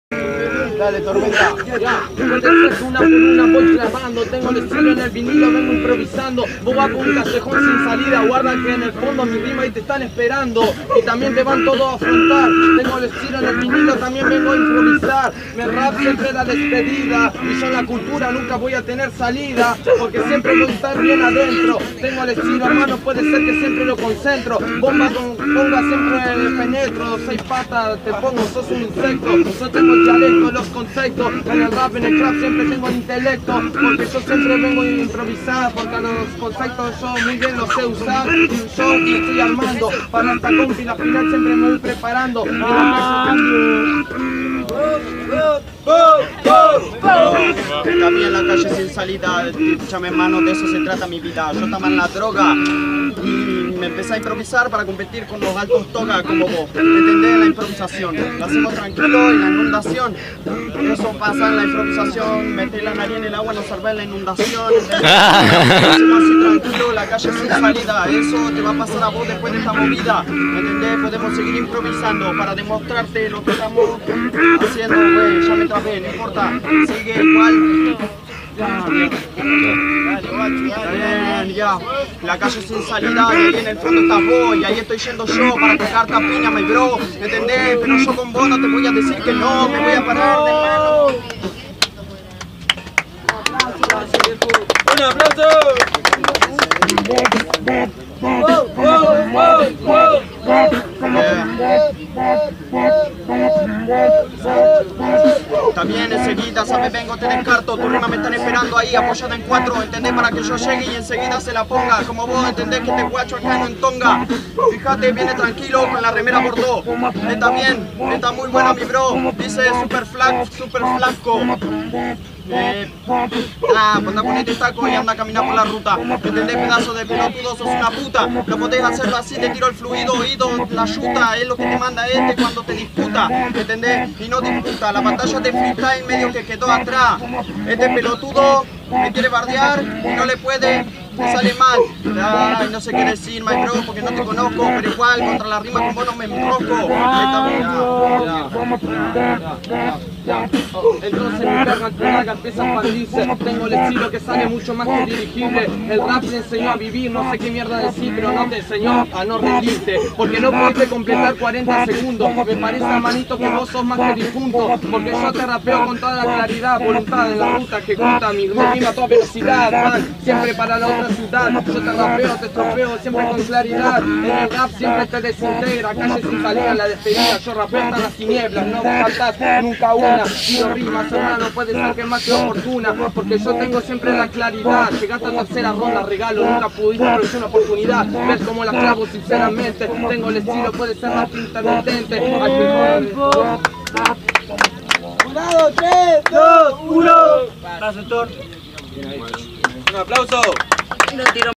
Storm vs Agus Semifinal Rap Clique 2da Edicion
Bueno , Salio la Segunda edicion de Rap Clique , se hace en la bajada del puente carretero (Santo Tomé)
Domingo 27 a las 17:30
1 vs 1 CON TEMÁTICA , Si hay replica es libre (SIN TEMÁTICA).
Compiten todos.
Jurados: Licho-Axel-Conec .